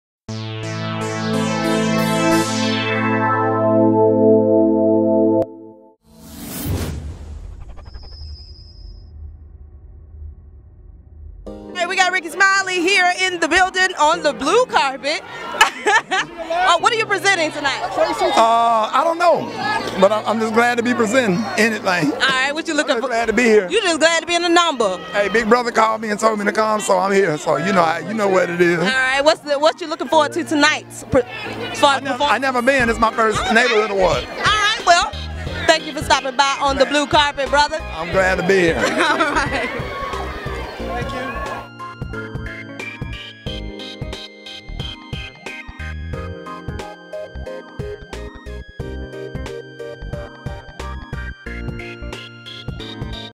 Neighborhood Awards 2014- Rickey Smiley
Isis chats with Rickey Smiley on the Blue Carpet during the 2014 Neighborhood Awards in Atlanta.

for more interviews from the red carpet, behind the scenes, and much more!!